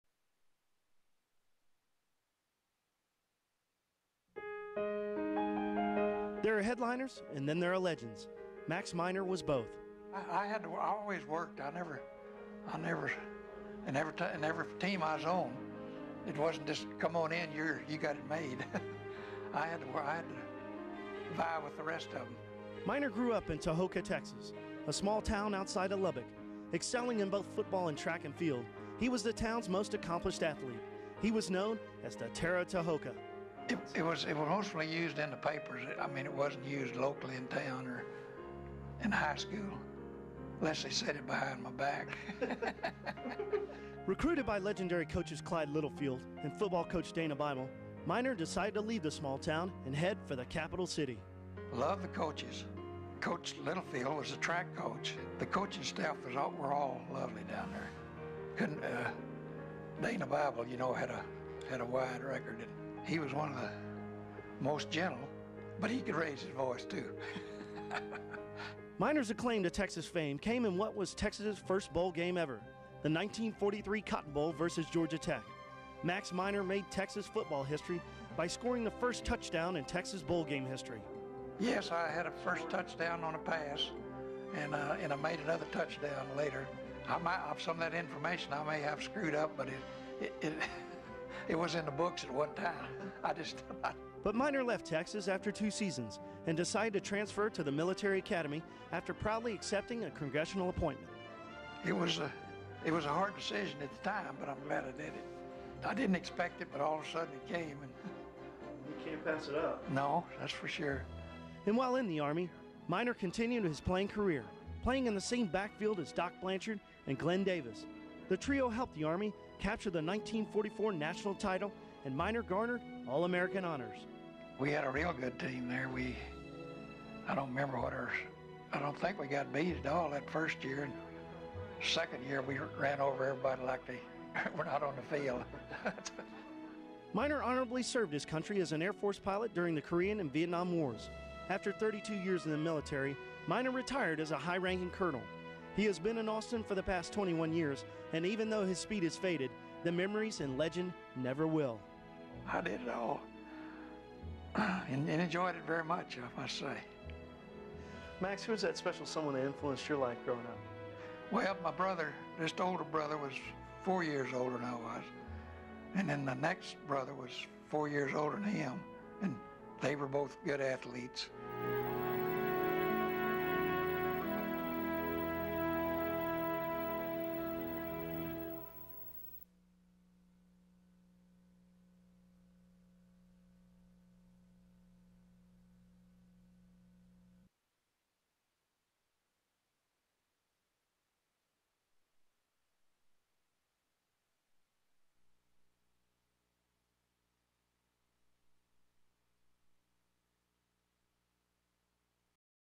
Max Minor KBVO
Max Minor talks about football @ UT and Army with Peter Gardere of KBVO TV in Austin, Texas, circa 2006